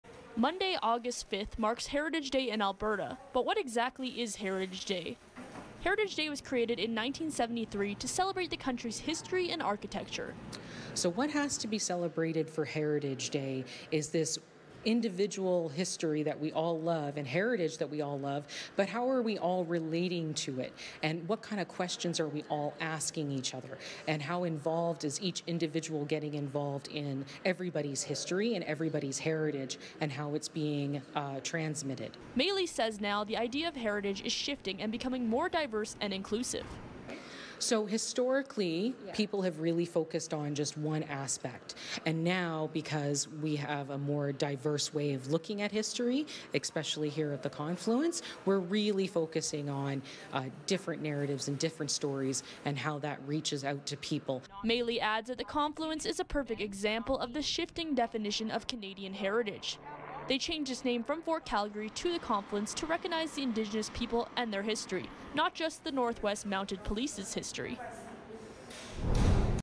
Defining Heritage Day in Alberta
Monday, August 5 marks Heritage Day in Alberta, but what exactly is Heritage Day?